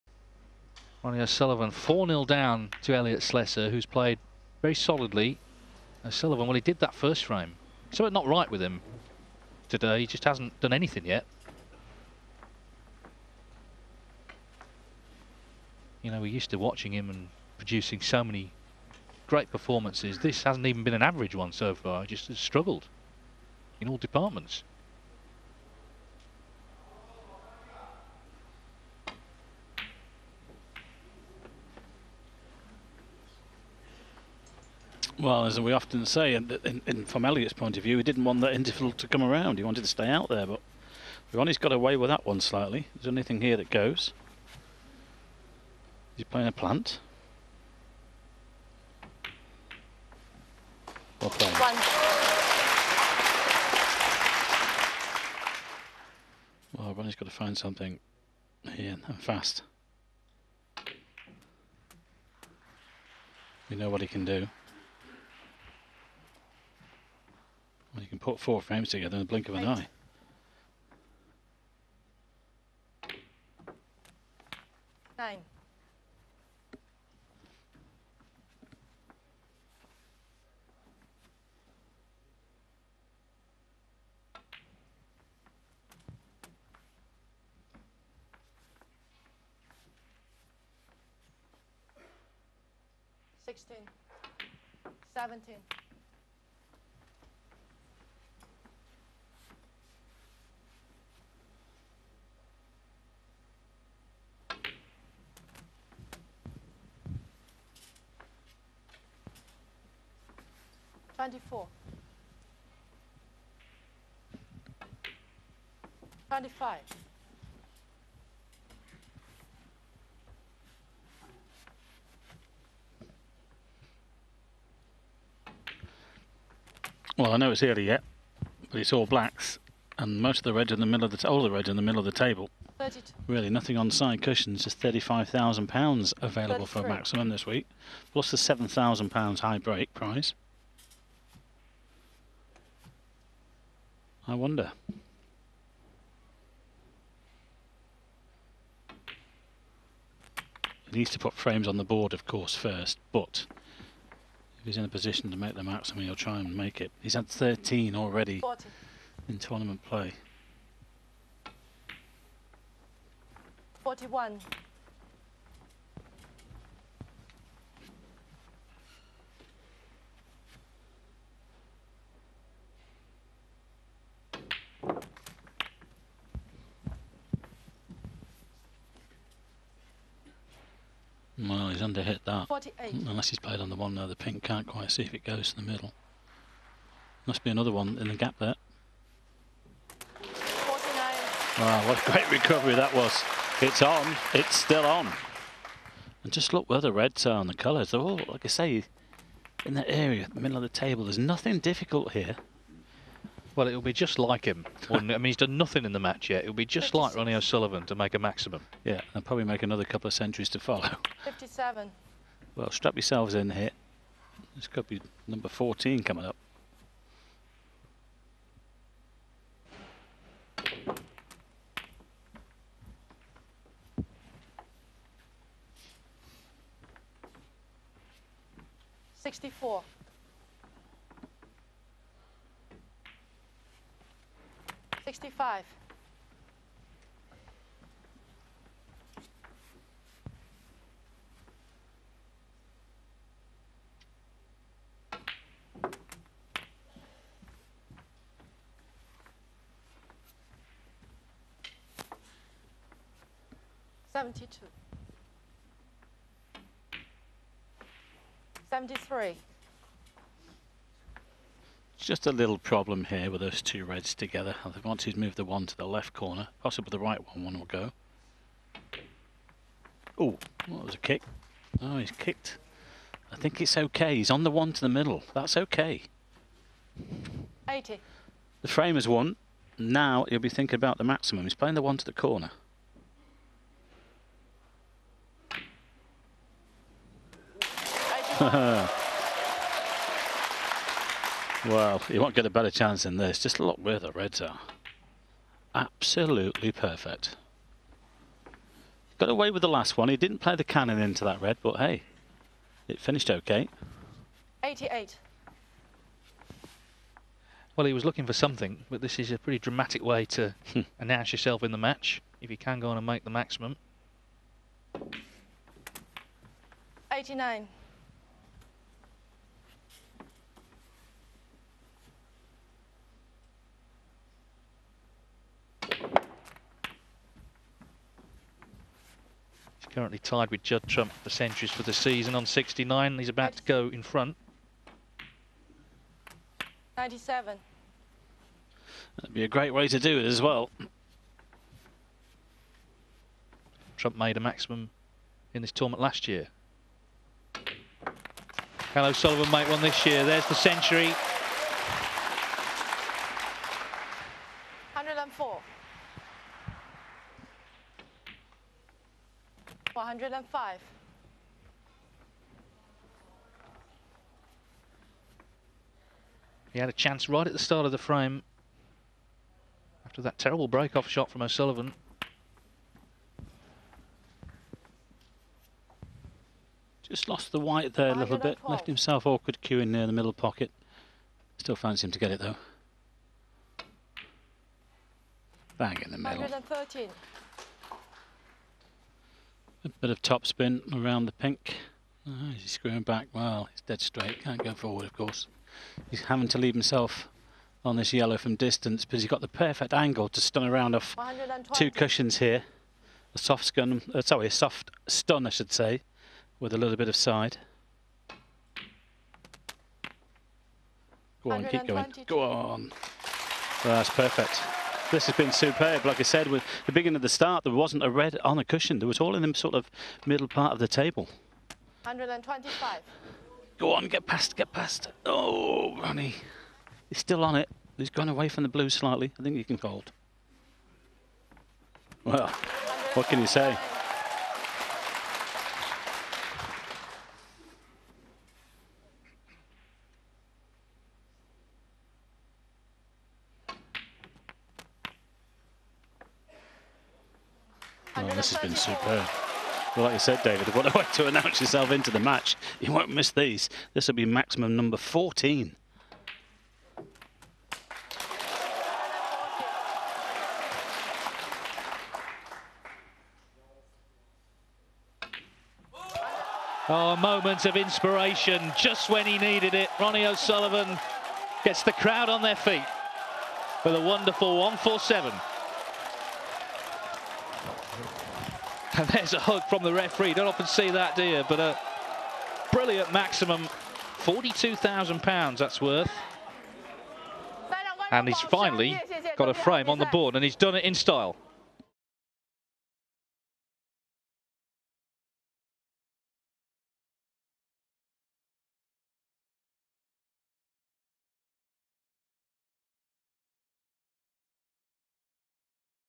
147 Classics: Ronnie O'Sullivan in China | Snooker | Eurosport
Remembering another Ronnie O'Sullivan's classic 147 against Elliot Slessor at the China Open 2018.